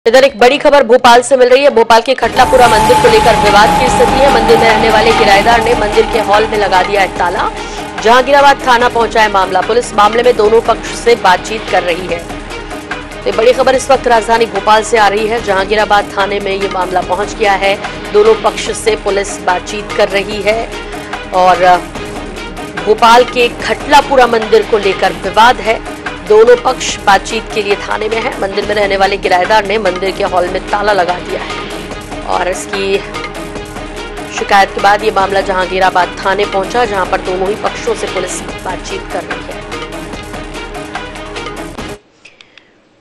Bhopal में खटलापुरा मंदिर को लेकर विवाद| मंदिर में रहने वाले किरायदार ने मंदिर के हॉल में लगाया ताला
INH, Bhopal में खटलापुरा मंदिर को लेकर विवाद| मंदिर में रहने वाले किरायदार ने मंदिर के हॉल में लगाया ताला

Incoming Search Terms: